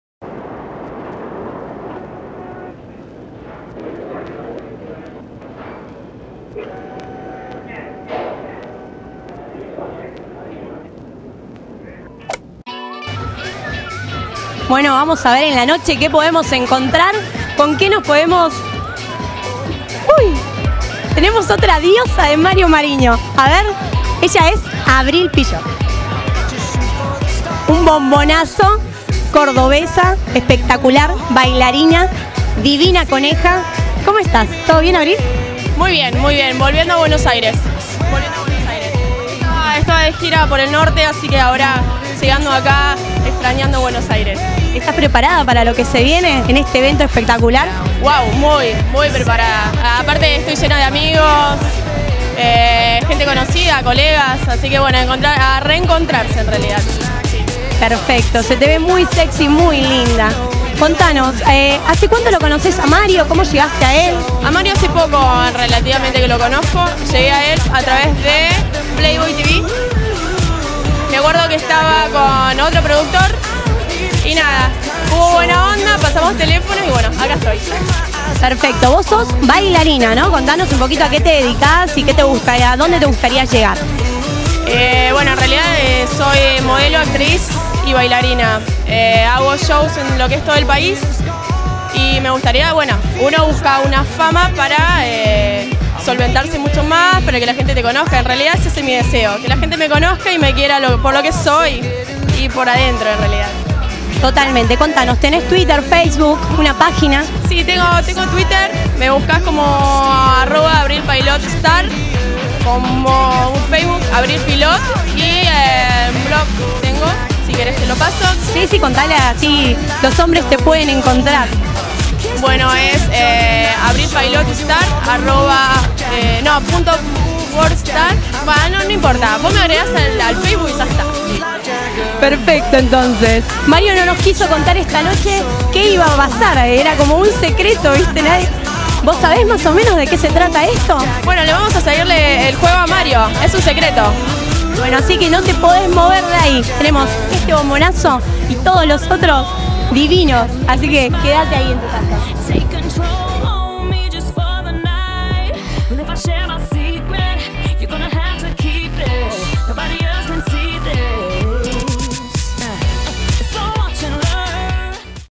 Last Night Party - Reportaje a Avril Pilot de MMSTAFF
Reportajes a las modelos de MMSTAFF en los eventos "LAST NIGHT PARTY " creados por Mario Marinho